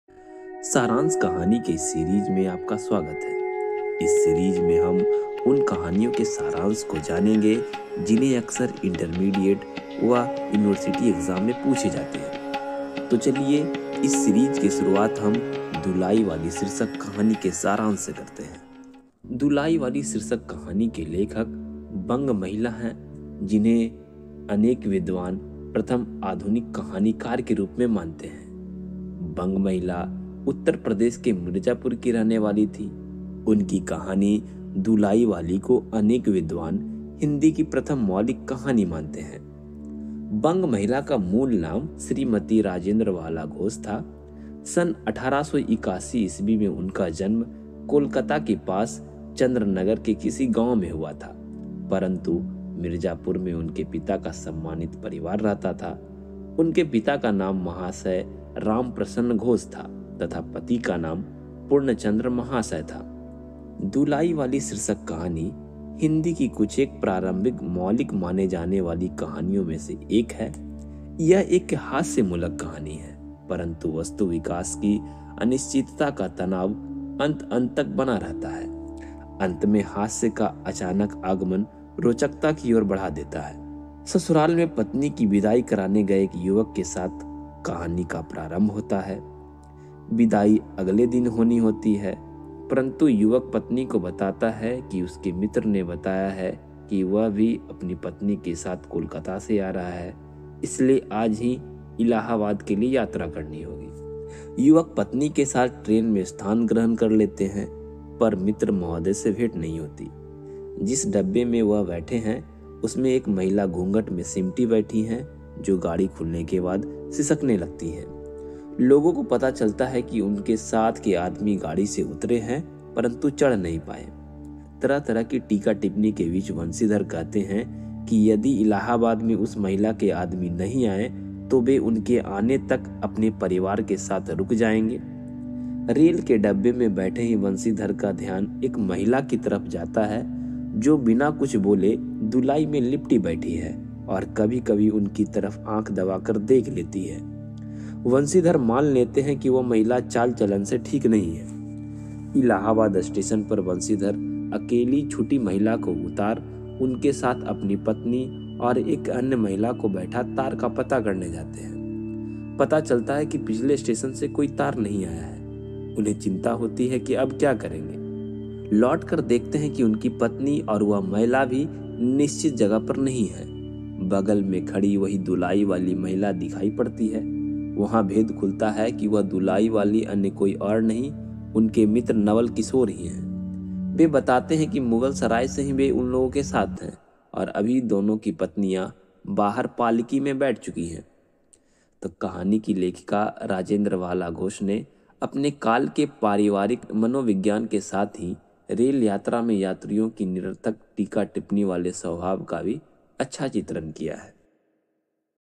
दुलाई वाली / Dulai Wali | Story Summary | Rajendra Bala Ghosh (Bang Mahila) | Hindi Composition
दुलाई वाली शीर्षक कहानी का सारांश | Smt. Rajendra Bala Ghosh (Bang Mahila) | Hindi Composition

This video is based on the summary of the story titled Dulai Wali written by author Rajendra Bala Ghosh (also known as Bang Mahila).  This story summary is often asked in 12th and university examinations, so it is very important from the examination point of view of students.

My previous videos are:-

Rajveer Gaurav